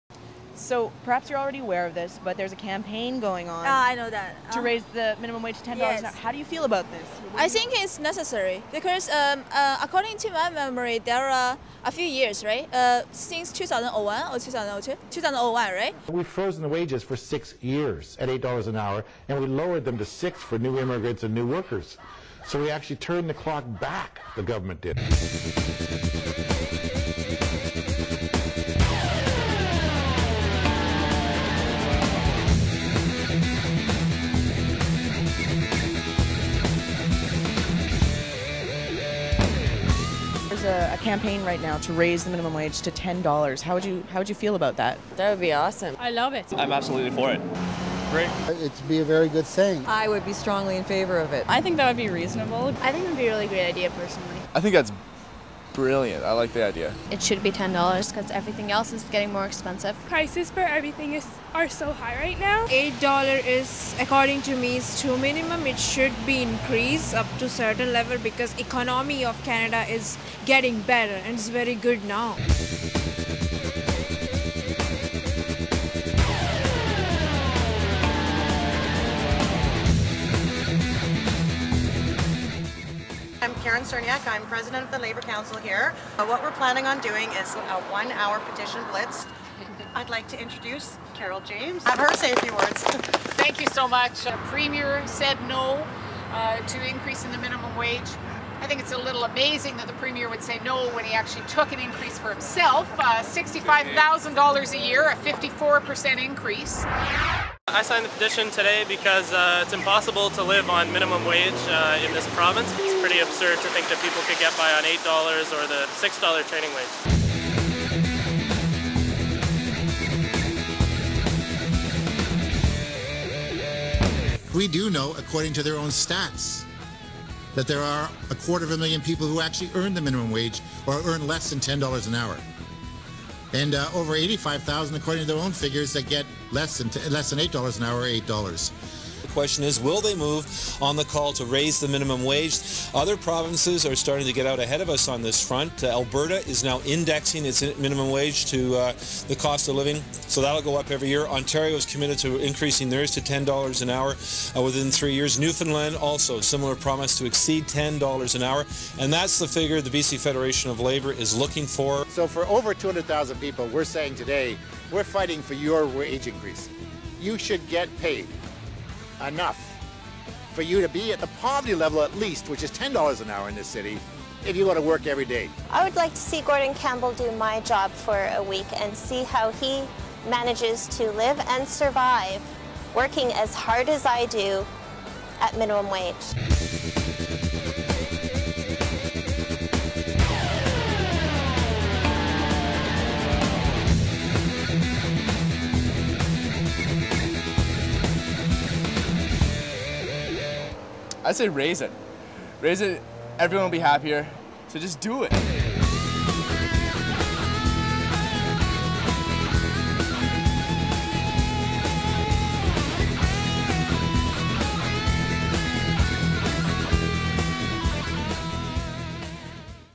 $10 NOW!
BC Fed President Jim Sinclair on the campaign to raise the minimum wage in British Columbia to $10/hr.